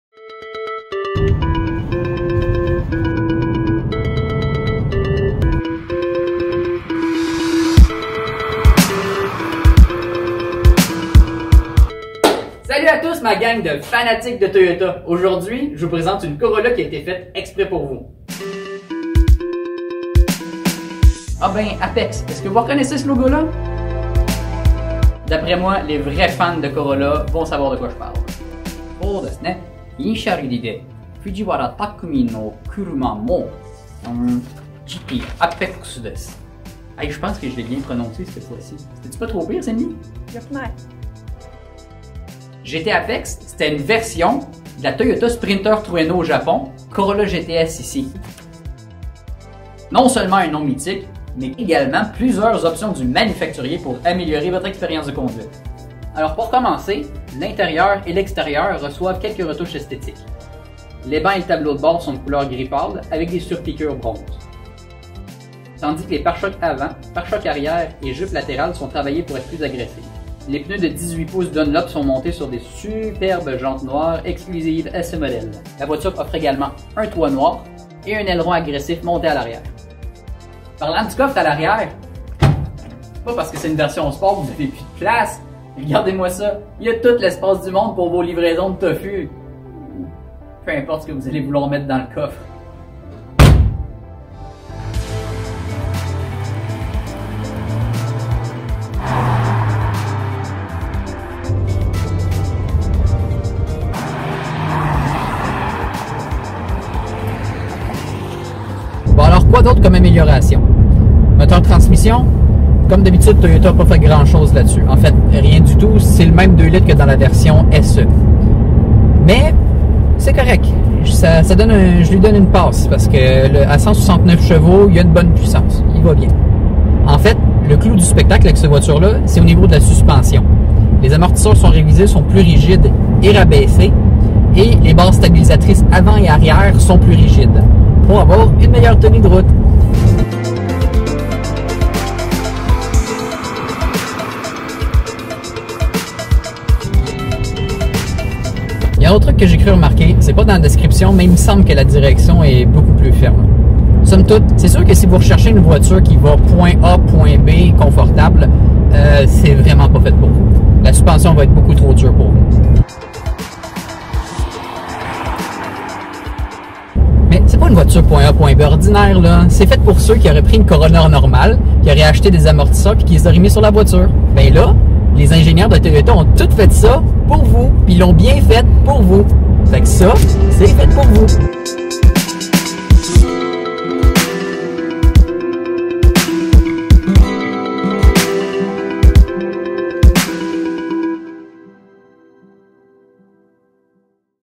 Toyota Corolla Apex 2021 à 2022 - Toyota St-Eustache
**Update, si vous regardez pour un modèle 2022, la présentation tient encore très bien la route; la plupart des caractéristiques du modèle sont encore les mêmes. Pour plus de détail, consultez l'un de nos représentant, ou allez directement sur notre site internet:

Dans cette vidéo, Philippe vous montre une nouveauté chez Toyota. C'est la Corolla Apex 2021. Cette édition limitée saura certainement plaire à un vrai fan de Toyota avec sa suspension rabaissée, sa meilleure tenue de route et avec son look plus agressif.

0:00 Introduction
0:48 Les extras
1:35 Impressions de conduite
2:33 Conclusion